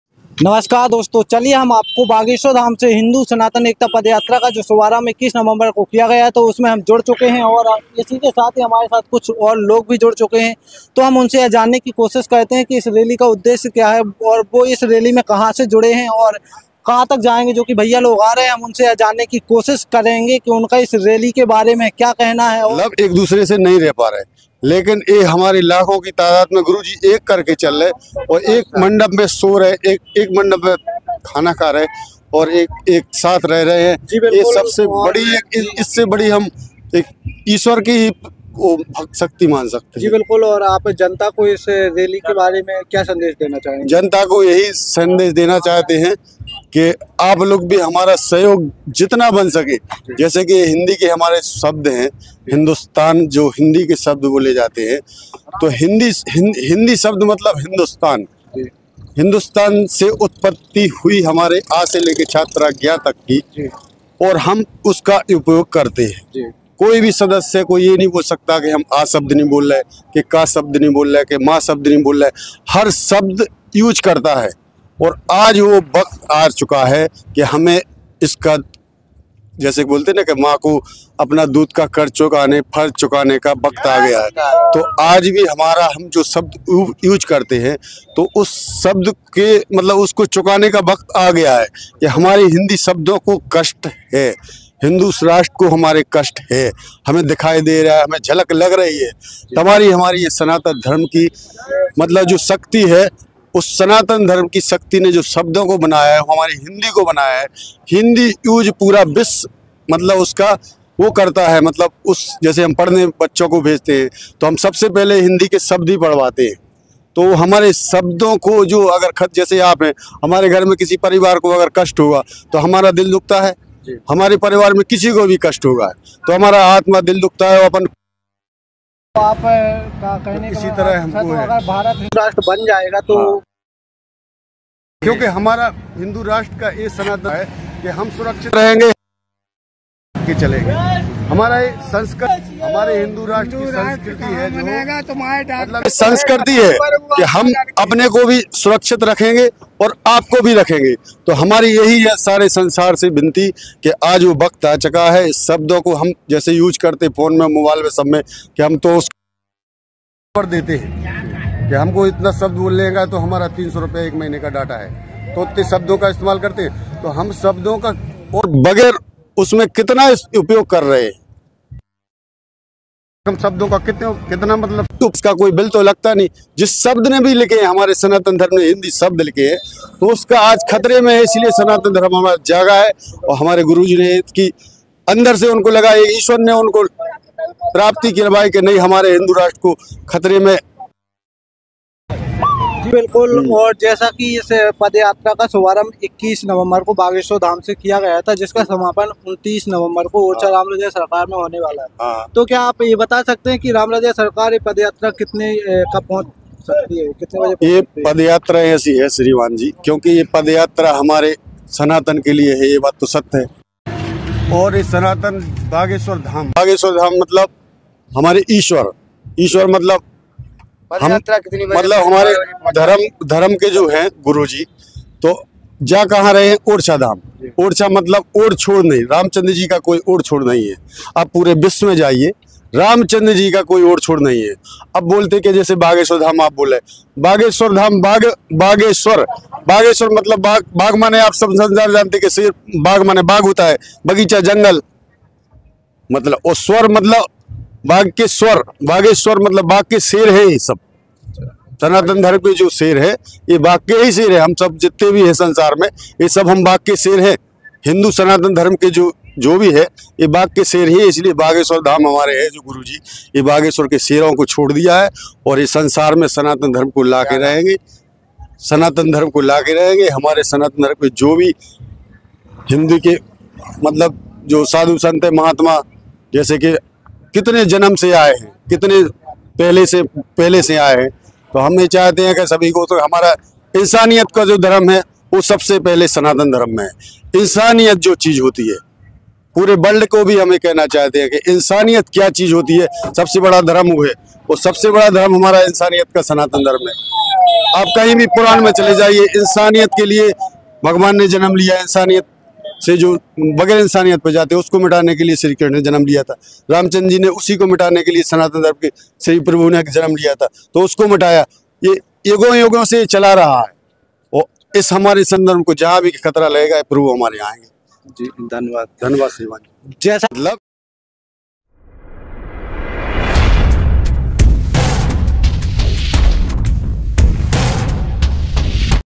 चलो बागेश्वर धाम से ओरछा धाम || हिंदू जोड़ो पदयात्रा || Sanatan Hindu Ekta padyatra
बागेश्वर धाम से ओरछा धाम जाने वाली पैदल यात्रा की आज तीसरे दिन की संपूर्ण जानकारी देखिए आज तीसरे दिन का रात्रि विश्राम महाराज जी कहां करेंगे और महाराज जी खाना कहां खाएंगे और कहां सोएंगे वह संपूर्ण जानकारी इस वीडियो के माध्यम से जानिए